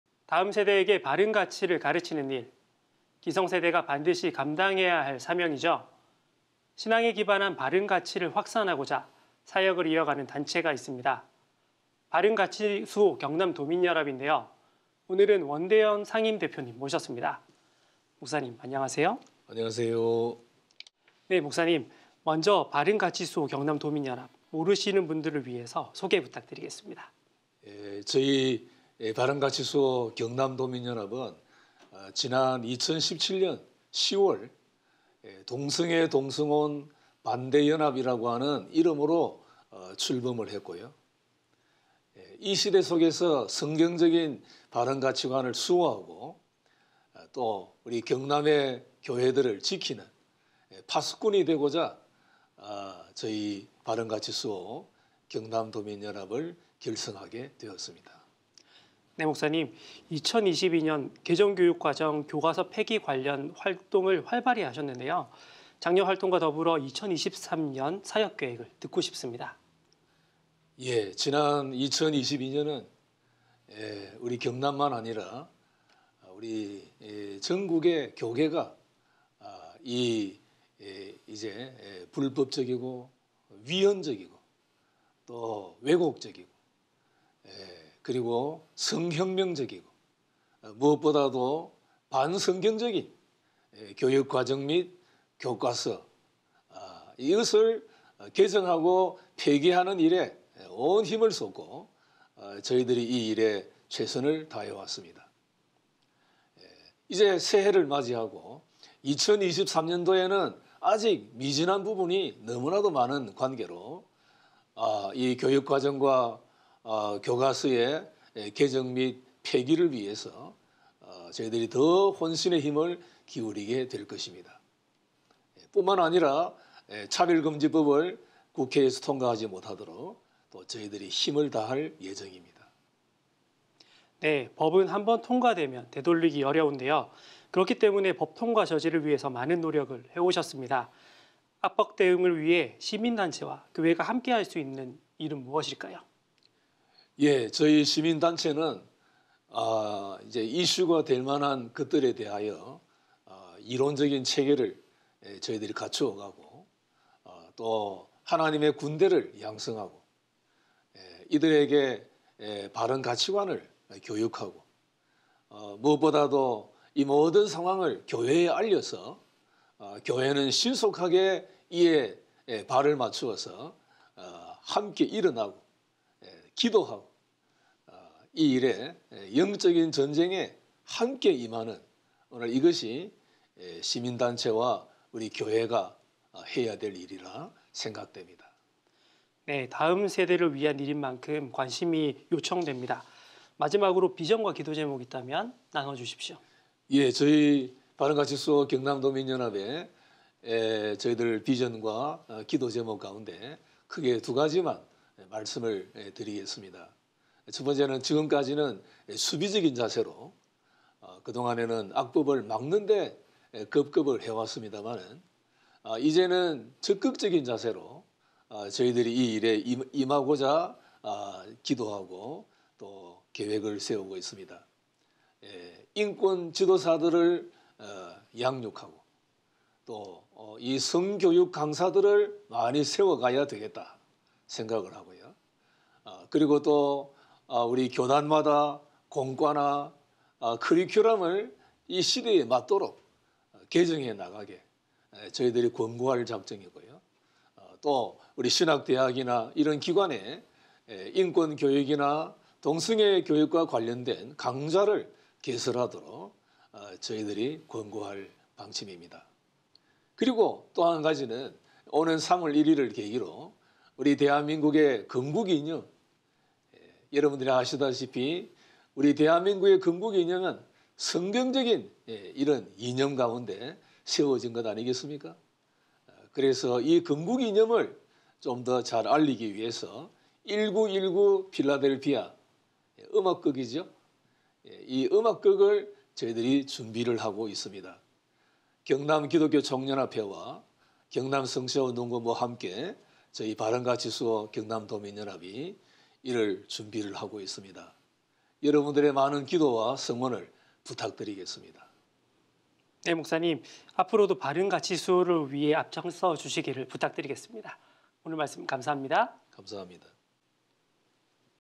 바른가치수호경남도민연합 상임대표, 마산교회 원대연 목사 l CTS뉴스
Q. 바른가치수호경남도민연합 소개
Q. 작년 활동과 올해 사역 계획은?
Q. 악법 대응을 위한 시민단체와 교회 역할?
Q. 비전과 기도제목